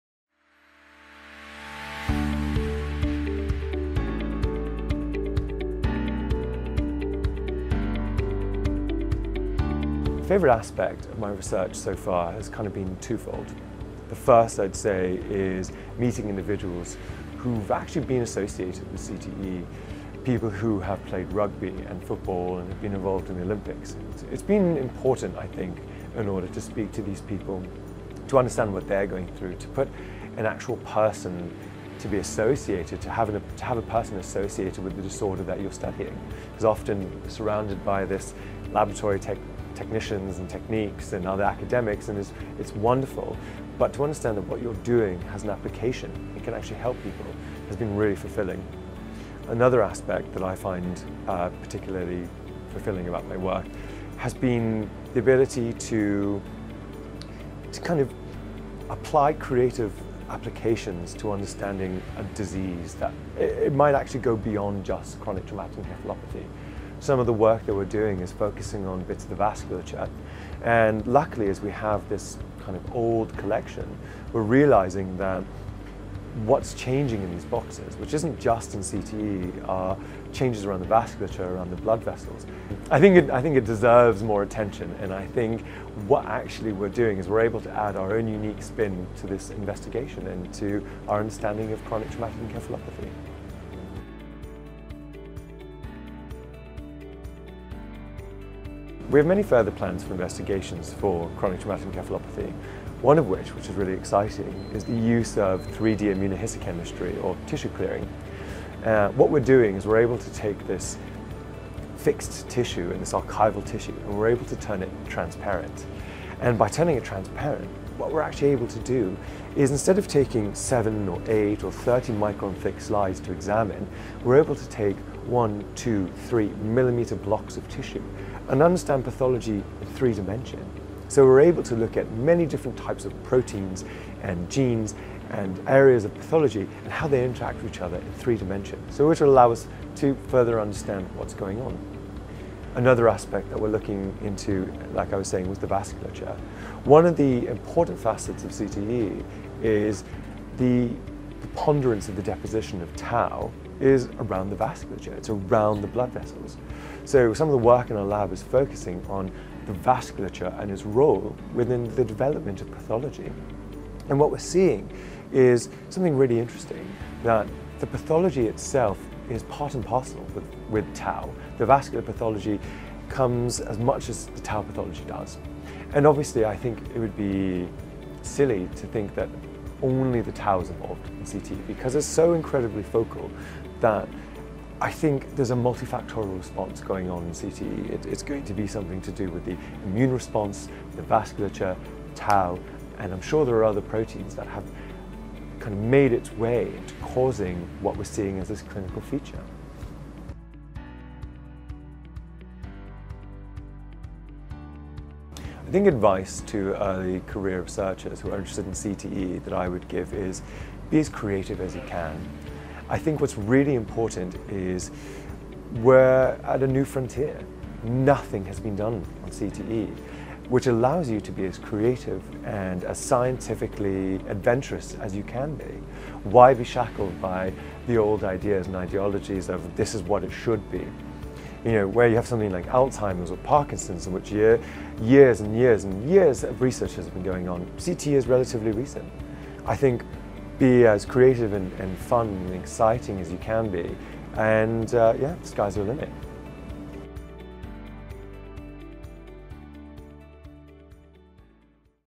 ‘Punch drunk syndrome’: an early career perspective with Marc Goldfinger
Chronic traumatic encephalopathy (CTE), formerly known as dementia pugilistica or ‘punch drunk syndrome’, is described as a progressive neurodegenerative disease in people with a history of repetitive mild traumatic brain injury. The symptoms of CTE are extremely similar to those seen in other forms of dementia, particularly Alzheimer’s disease (e.g., cognitive impairment, memory loss, Parkinsonism, increasing confusion and disorientation, and slurred speech). As these clinical symptoms are shared with other neurodegenerative diseases, there’s currently no available test to diagnose the condition. At present, the most reliable way of confirming diagnosis of CTE is by examining post mortem brain tissue for certain changes.

To find out more, we spoke with Marc Goldfinger (Imperial College London, UK) about the disease. In Part 1 of the video, Marc provides us with a description of CTE, as well as a pathological demonstration illustrating the changes observed in post mortem brain tissues of individuals with CTE. He also talks us through his work with boxer’s brains, which focuses on a particular collection called the ‘Corsellis collection’.

In this second part, Marc describes what his favorite aspect of his research has been so far, including further investigations he has planned. Marc also provides advice for early-career researchers looking to study CTE.